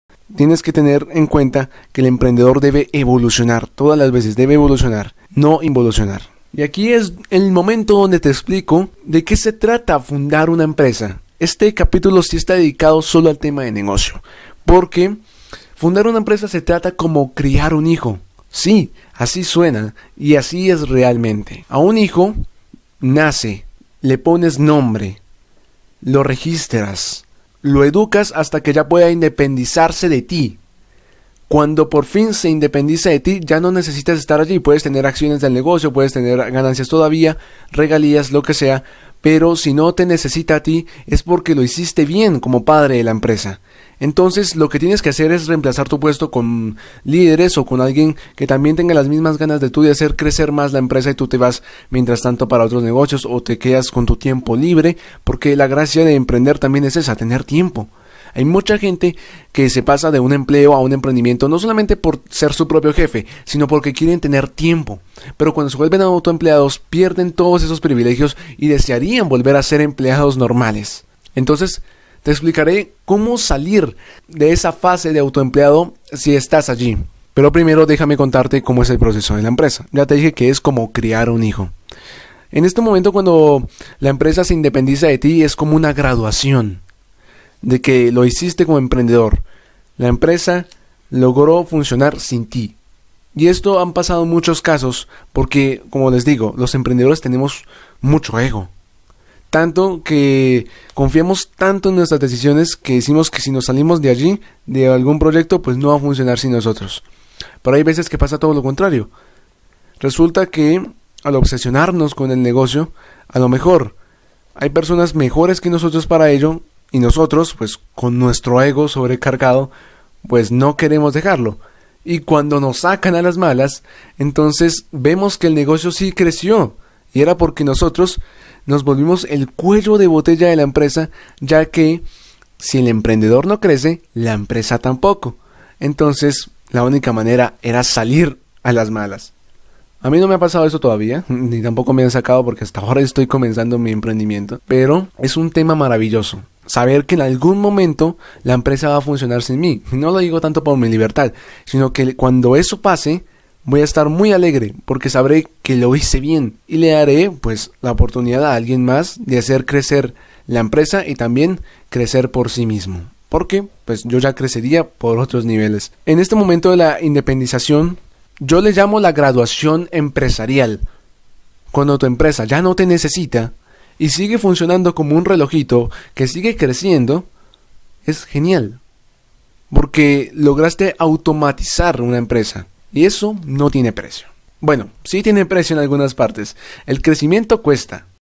¡DE AUTO-EMPLEADO A EMPRESARIO! | EFECTO DISRUPTIVO
Si el emprendedor no crece sus proyectos tampoco, cuando te auto-empleas en tu propio negocio pierdes una gran parte de libertad, ahora toda la responsabilidad cae sobre ti, entonces ¿como obtengo la libertad que he estado buscando?
¡Presta atención Escucha el podcast!

Tu tiempo aquí bien vivido es tu mayor recompensa.